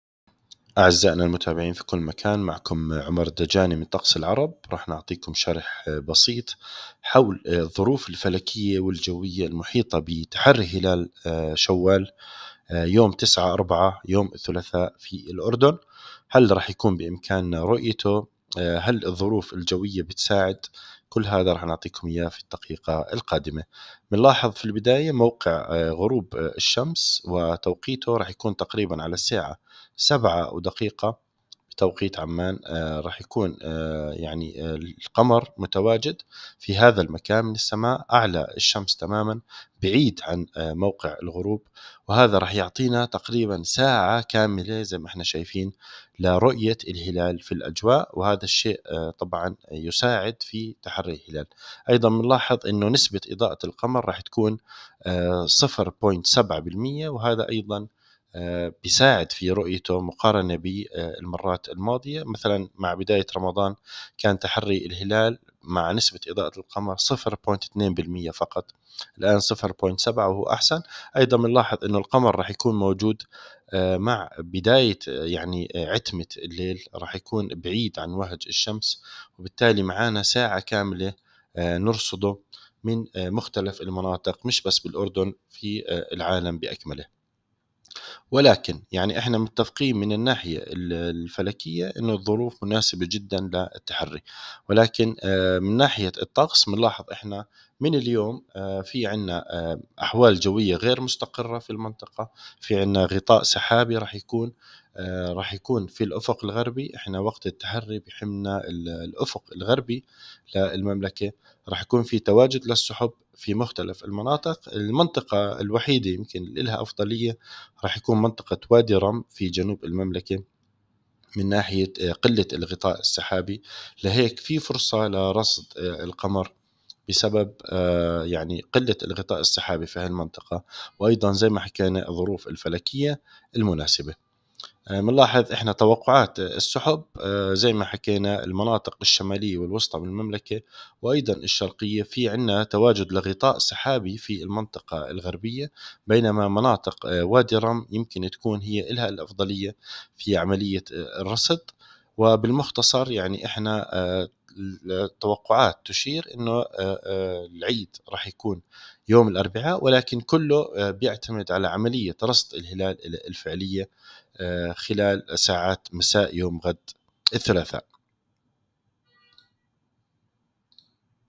الظروف الفلكية والجوية المحيطة بتحري هلال شوال يوم 9 أبريل في الأردن
شرح بسيط حول الظروف الفلكية والجوية المحيطة بتحري شهر شوال اليوم 9 أبريل 2024.. هل سنتمكن من رؤيته ؟ وهل الظروف الجوية ستساعد على ذلك؟ تعرف على الجواب مع الزميل عمر الدجاني ! 📢🔭